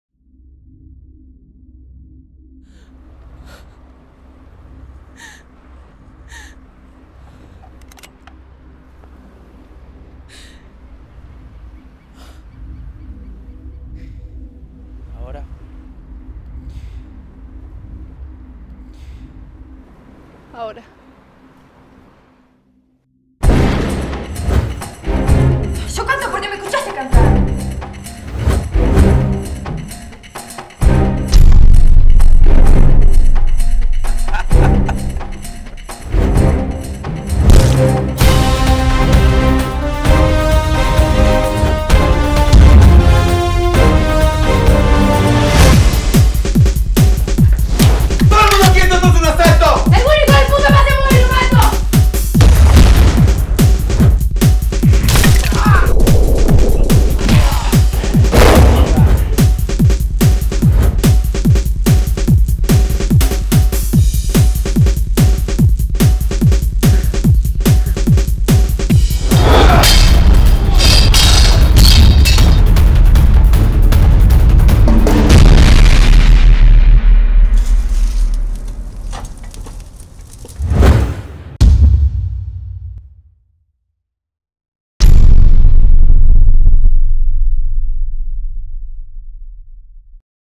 Spot Curso de actuacion 2011 Escuela de cine Dodecá
Curso de actuación para cine

El curso es una introducción y un entrenamiento en la actuación en cine. Se realizarán ejercicios y escenas frente a cámara y con grabación de sonido. La propuesta es introducirse en la actuación como disciplina dramática y dentro de las especificidades de la actuación para cine. Se brindarán conocimientos de combate escénico a los interesados, la posibilidad de participar en un rodaje final, y actuar en un cortometraje de la escuela de cine dodecá, sujeto a la demanda de los estudiantes del curso de cine.

Docentes: Belén Baptista, Valentina Martinez y Santiago Ventura